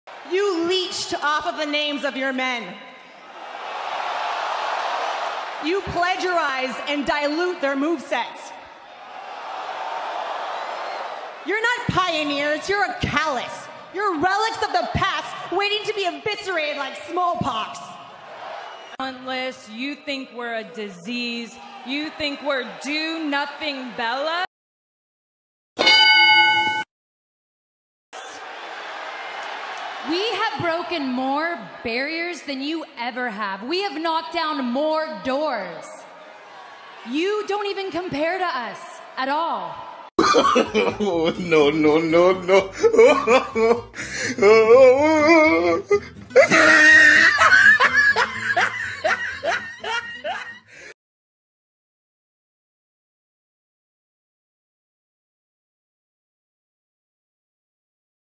The Bella Twins Secret To Longevity In The WWE
Why does the WWE always bring the Bella Twins back? Find out their secret to longevity in the WWE for all these years.